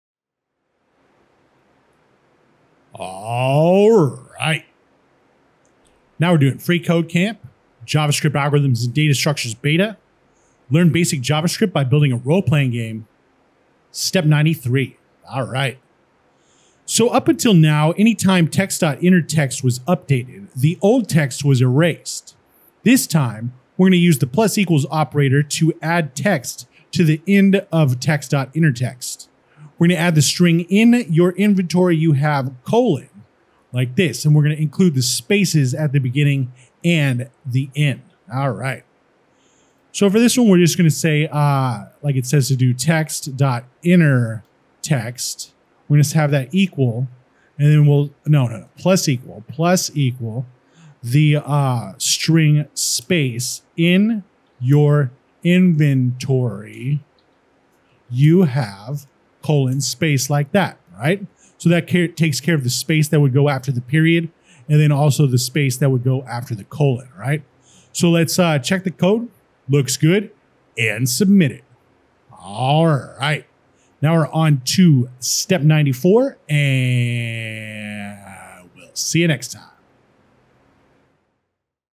(ARCHIVED) Learn JavaScript by Building a Role Playing Game: Step 93 | freeCodeCamp
🌟 **"Enhancing Game Narratives: Updating UI Text in RPG - Dragon Repeller"** 🌟

Elevate your RPG - Dragon Repeller game by skillfully appending new text to existing UI elements. This tutorial is about more than just adding text; it's about creating a fluid and evolving narrative display that dynamically responds to player actions.

🔢 **Dynamic UI Text Updates with the += Operator**:

Learn the art of seamlessly adding new content to existing text in your game's UI. This tutorial will guide you through using the `+=` operator to append the phrase "In your inventory, you have: " to the `text.innerText` property. Understand how this technique allows for an ongoing narrative that builds upon previous actions, enhancing the player's sense of progression and immersion in the game world.

📚 **Hands-On Exercise: Crafting Cumulative UI Narratives**:

Join us in a practical session where you'll bring a new level of storytelling to your game's interface. Ideal for those looking to enhance their JavaScript skills, this tutorial focuses on augmenting the `text.innerText` property with additional descriptive text. You'll learn how to use the `+=` operator to create a continuous and engaging narrative that reflects the player's journey and inventory status.

Each lesson, including this one on dynamic text updates, is crucial for creating engaging and responsive web games. By liking this video and sharing it with others, you support a community passionate about innovative storytelling and interactive game design.

📜 **Begin Your Adventure in Dynamic Narrative Design**:

Embark on the 'Enhancing Game Narratives: Updating UI Text in RPG - Dragon Repeller' challenge and share your experiences with implementing cumulative text updates in the comments. Let’s foster an engaging learning environment where we can explore and excel in the art of creating dynamic and evolving game narratives.